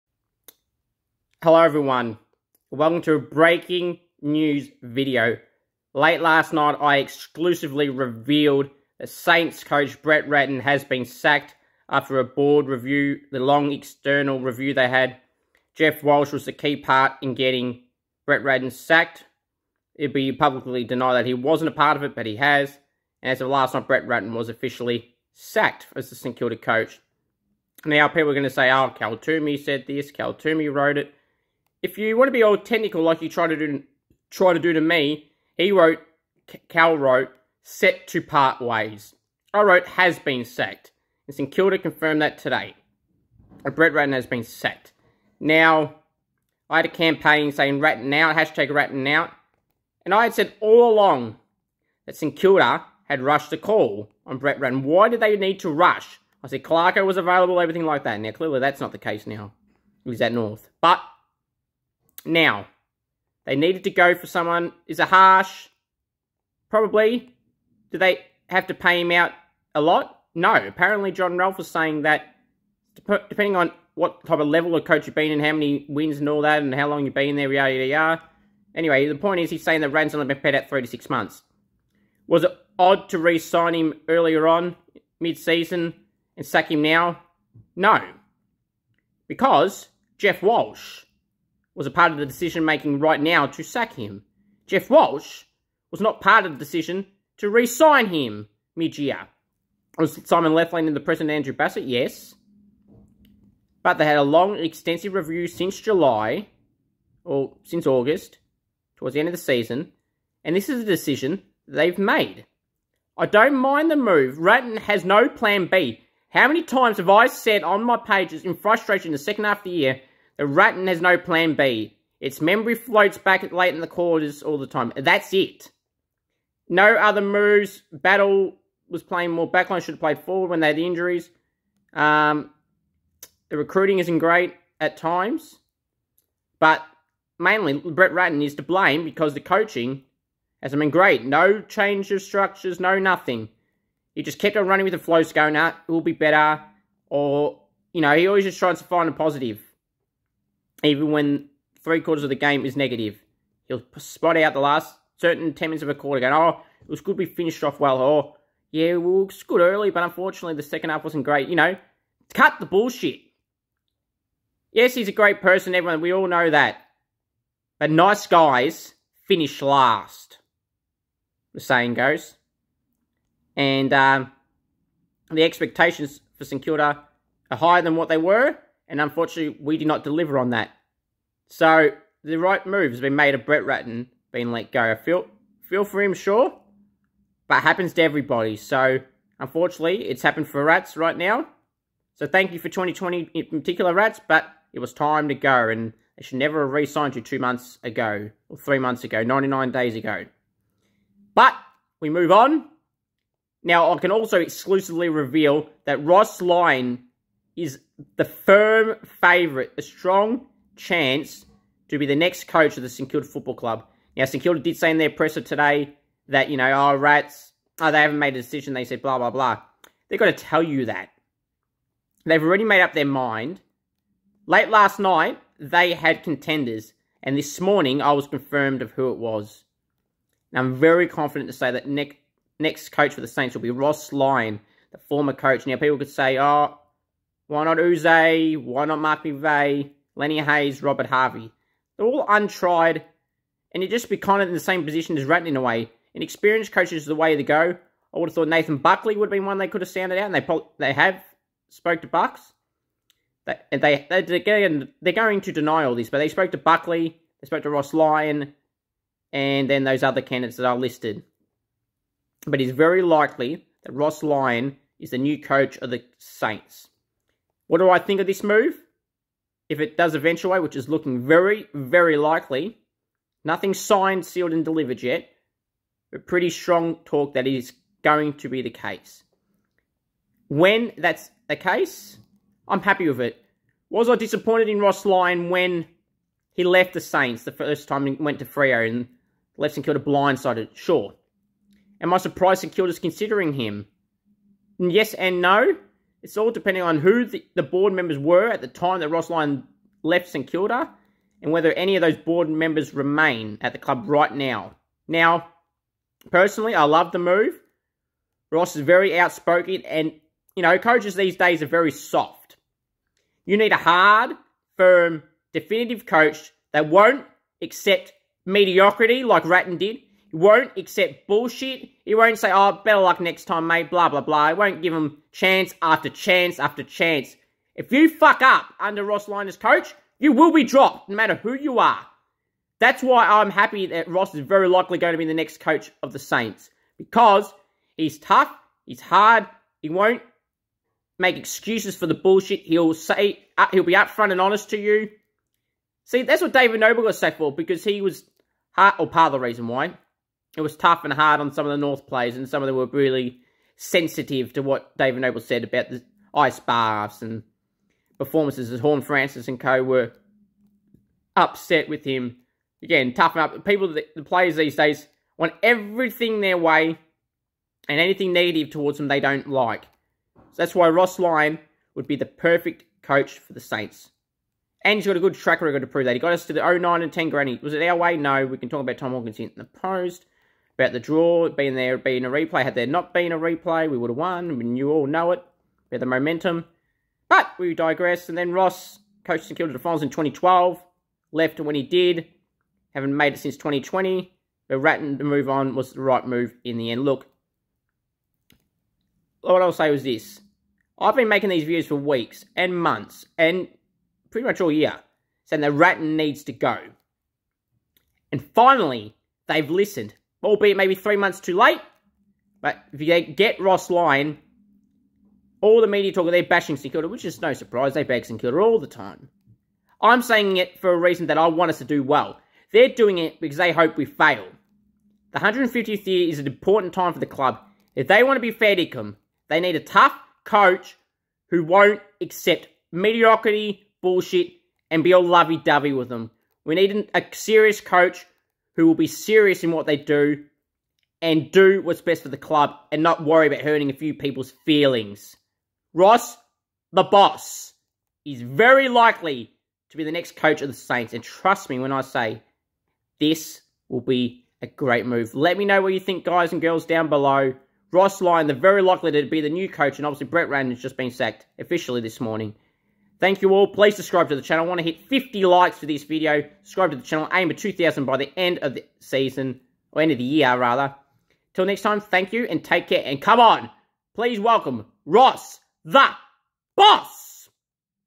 SAINTS COACH BRETT RATTEN SACKED. Ross Lyon returning?
My immediate thoughts of Brett Ratten being is sacked is the right decision and Ross Lyon to replace him?